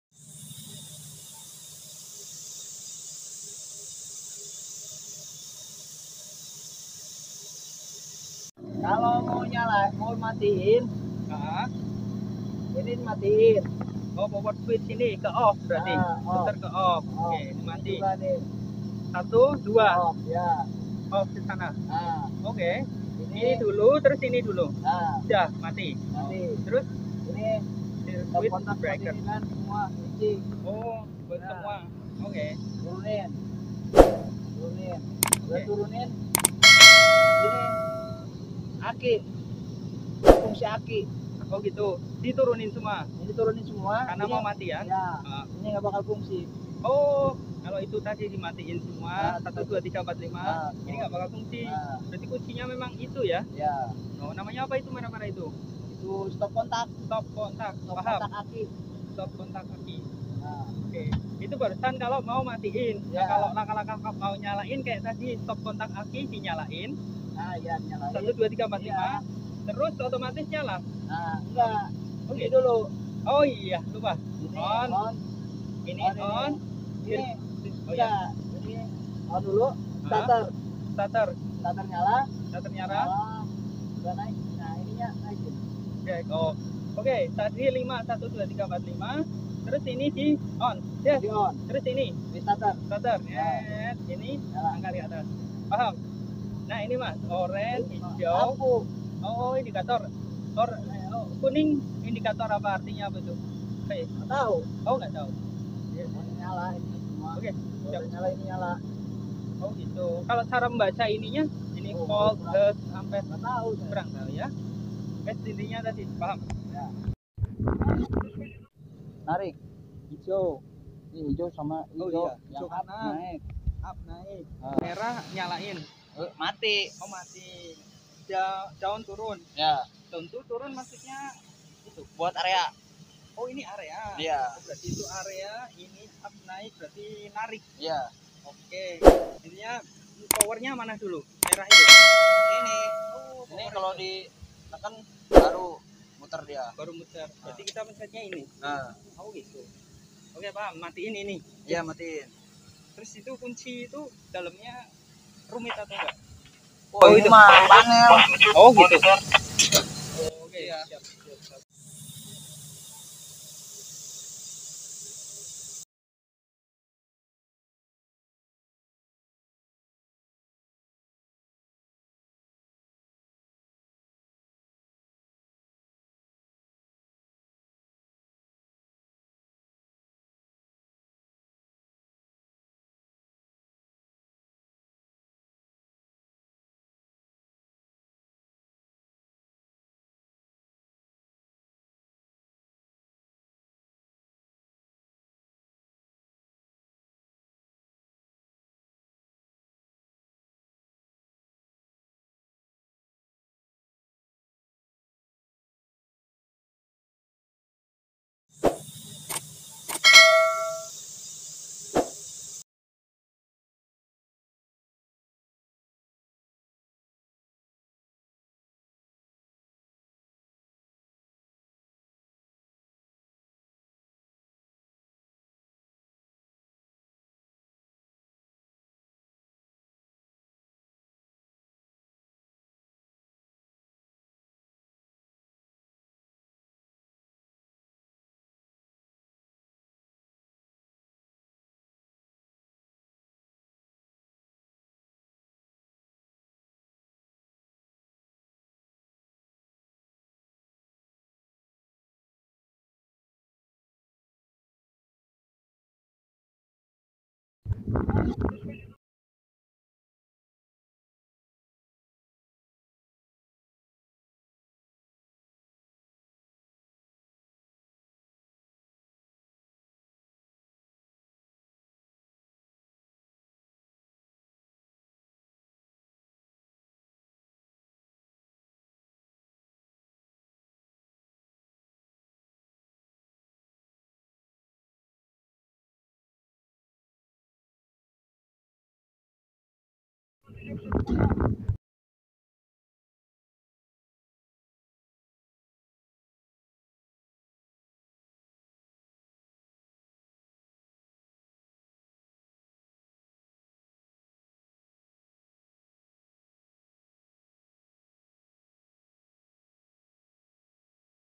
Tutorial Cara Mengoperasikan Genset HARGEN dan Winch untuk Tongkang
Ki Darus adalah Juru Kunci Tongkang sekaligus sesepuh kami yang sudah lama mendiami Bahtera Besi tsb. Berikut adalah hasil wawancara Saya kepada Beliau mengenai Cara Mengoperasikan, yakni Menghidupkan dan Mematikan Genset merek Hargen dan Winch untuk Tongkang.